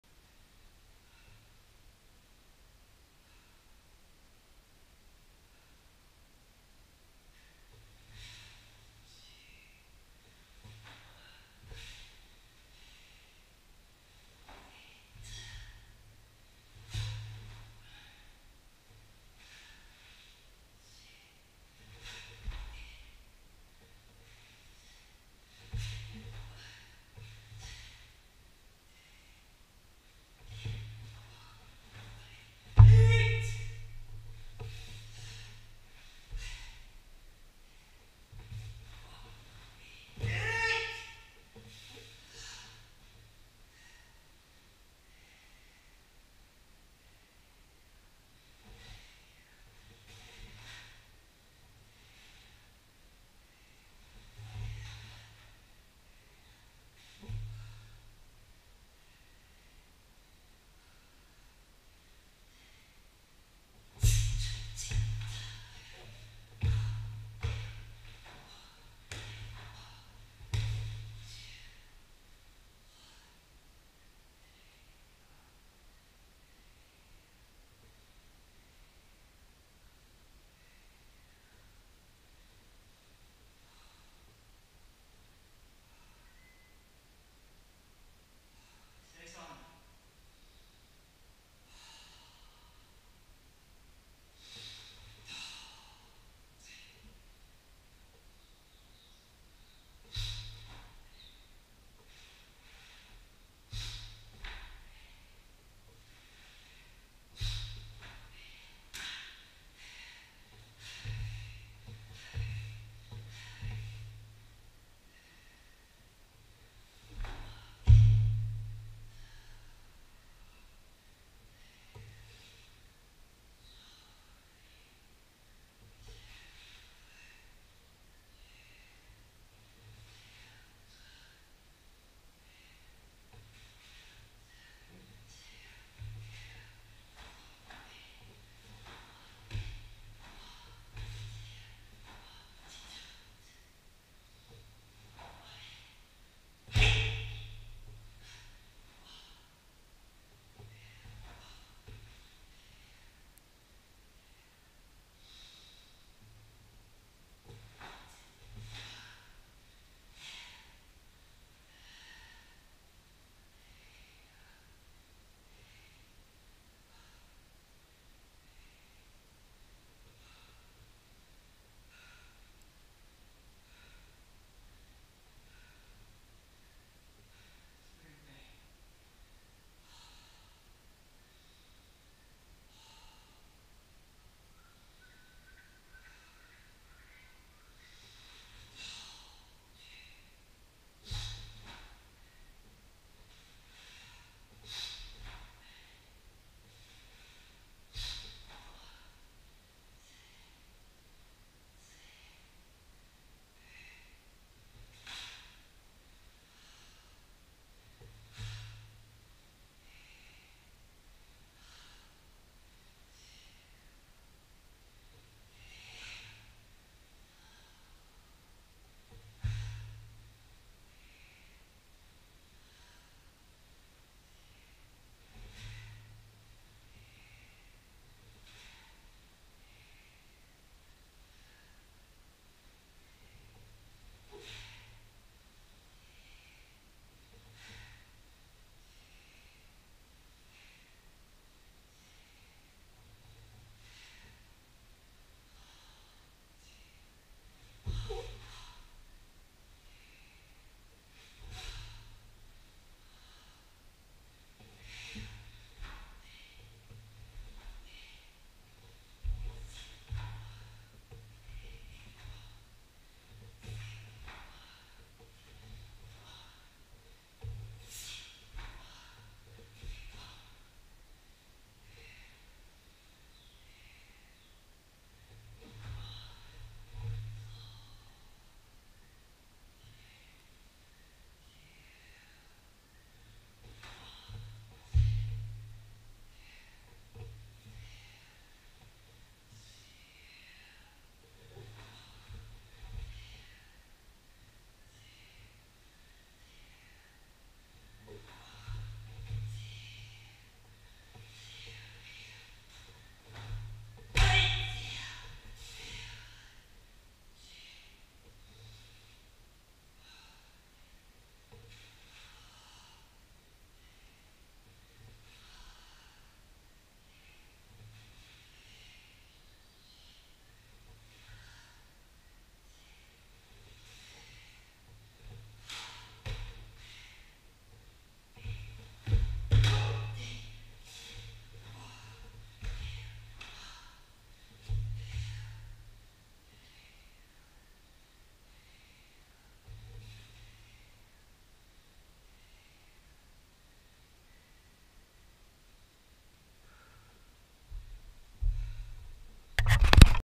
KURARUMFA,SESAN AND SUPARINPEI
AGE68: solo practice everyday...learned these katas a few months ago...every morning Karate Kobudo and KYUDO